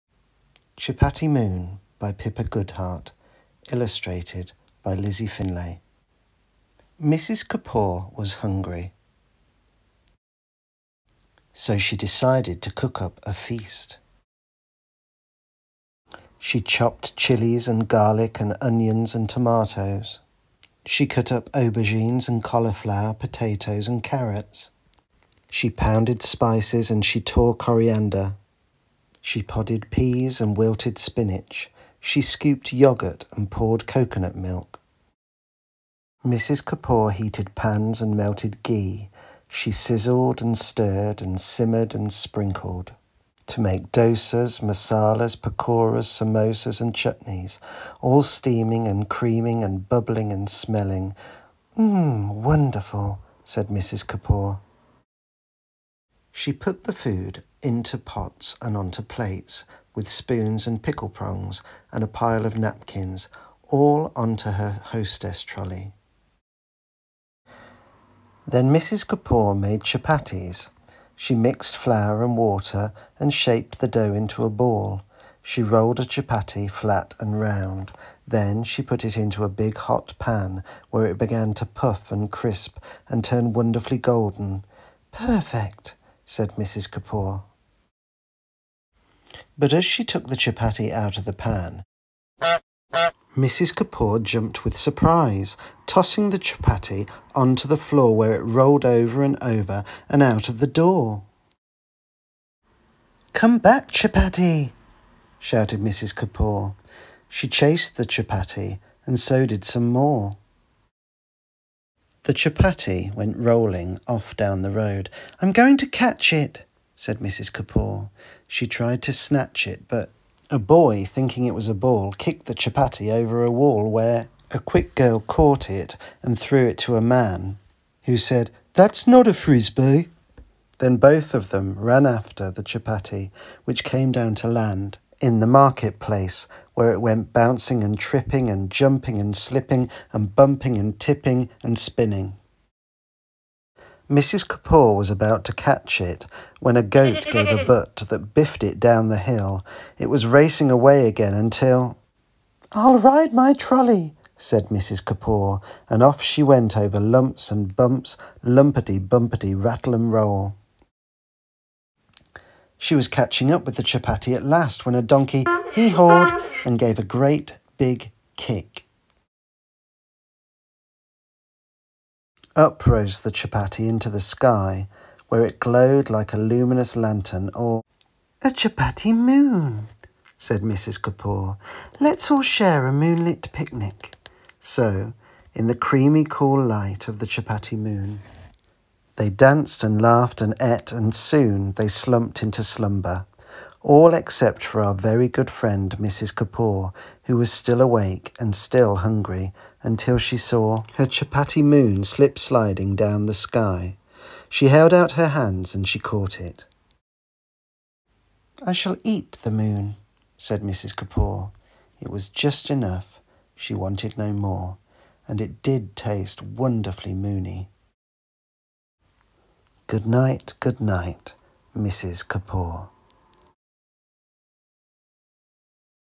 Chapatti Moon
Mr Loughlin reads Chapatti Moon by Pippa Goodhart and illustrated by Lizzie Finlay